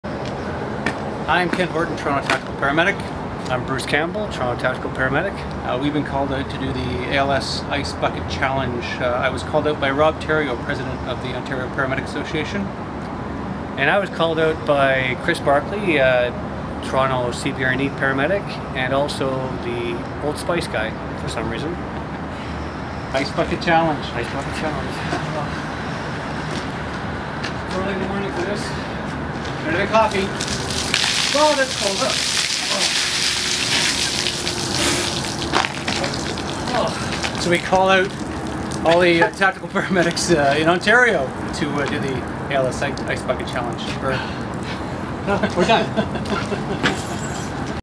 Bruce Campbell and Ken Horton take the challenge
Bruce Campbell and Ken Horton respond to the ALS ice bucket challenge from Ontario Paramedic Association president Rob Theriault  - Toronto CBRN medic Chris Barkley - and the old spice guy -